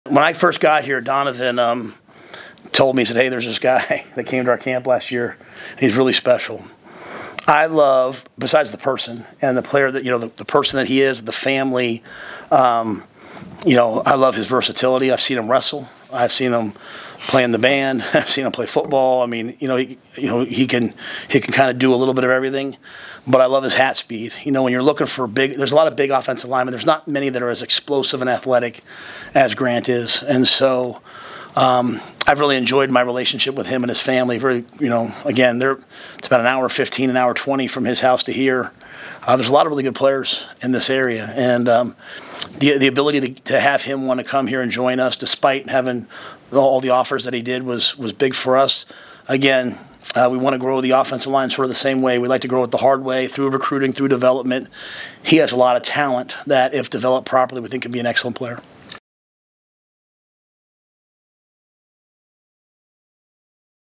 Signing Day 2023: Nebraska's Matt Rhule on Grant Brix
Nebraska's Matt Rhule talks about incoming recruit Grant Brix during his signing day press conference on Dec. 20th, 2023, in Lincoln.